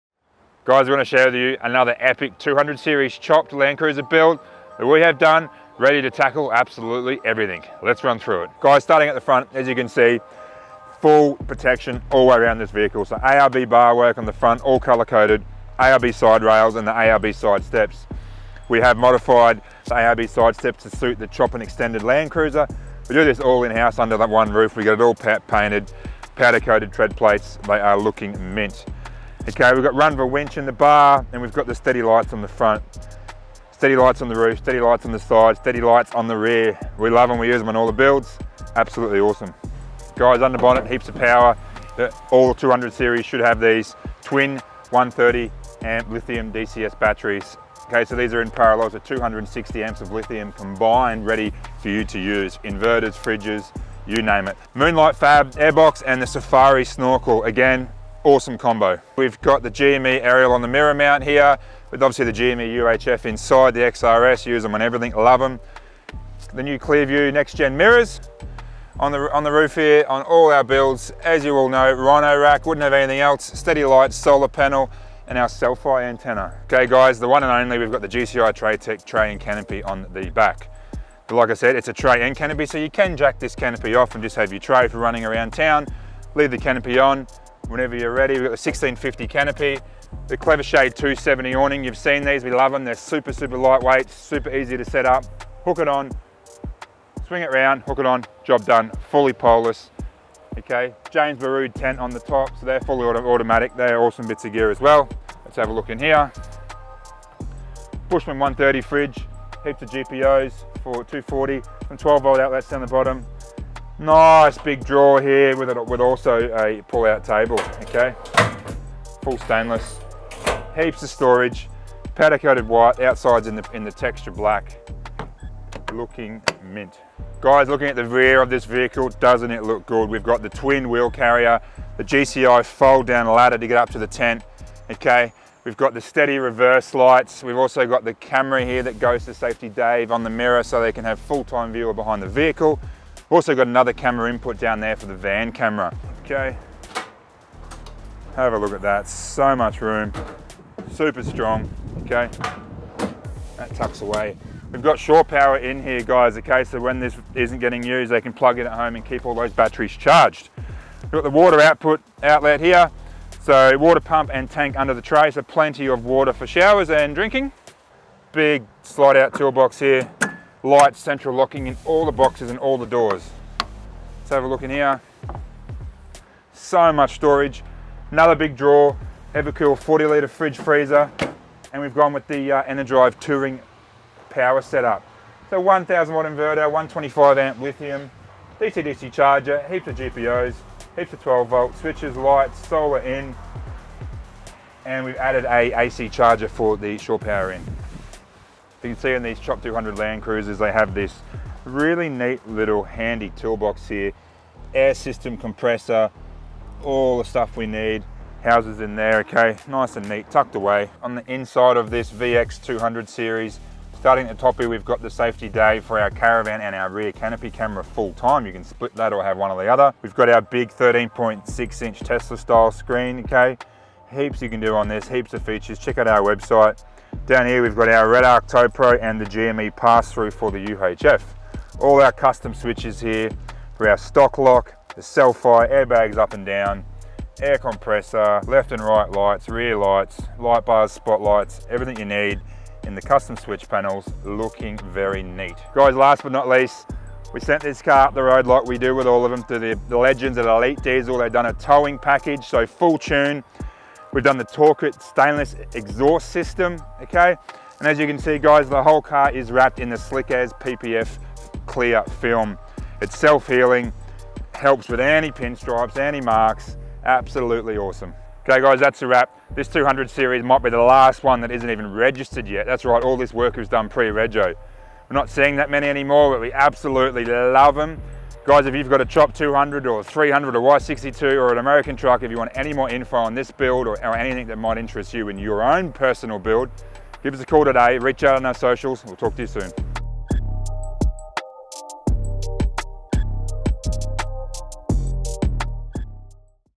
Last Modified 200 Series Landcruiser - CHOPPED Landcruiser & GCI Traytec tray canopy
Last Modified LC200 - CHOPPED Landcruiser & GCI Traytec tray canopy

Check out what would have been the last modified 200 Series Landcruiser

See our quick run-through we did on one of the last chopped VX 200 Series Landcruiser.

We have decked this out with just about everything you can imagine. If you're thinking about decking out your 200, get in contact with us